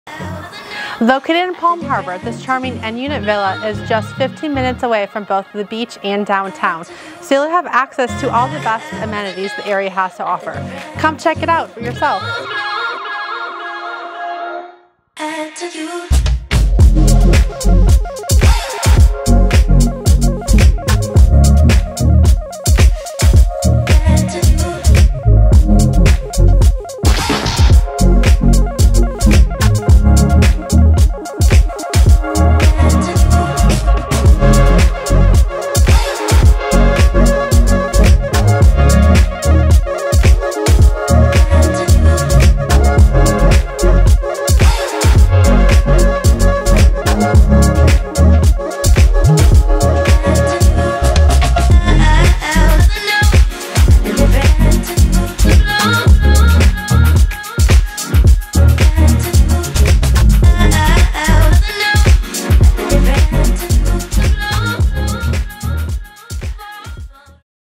300 Buckingham Pl., Palm Harbor, FL MLS# T3427980
This charming 2 bedroom, 2 bath, 1 car garage end-unit VILLA in Palm Harbor, FL is low maintenance and the perfect place to call home! Located in the highly desirable Steeplechase subdivision off Tampa Rd in a NON-EVACUATION Zone, just a short 15-minute drive from both the beach and downtown, you'll have access to all the best amenities the area has to offer. The spacious one-story floor plan features a huge backyard, perfect for outdoor entertaining and relaxing in the Florida sunshine. It has a new roof, new HVAC, new Levinton electrical outlets, switches and fans, a fully renovated kitchen, updated bathroom lighting, fresh paint throughout the home, and the dining room chandelier has recently been changed. Upon entry, you are greeted by an inviting foyer that opens flawlessly into a very spacious and open-concept living room. The split floor plan allows for needed privacy for both bedrooms and creates separate living quarters. Enjoy ample space for eating dinners as a family from the overhanging bar to the kitchen nook, and large dining room. The main living space, kitchen, and primary bedroom all have access to the enclosed patio to truly enjoy Florida living. No need to worry about your power going out, it's hooked up to the emergency system for Arden Courts Assisted Living facility behind the community. With its prime location and charming features, this townhome is a must-see for anyone looking for a comfortable and convenient home in Palm Harbor!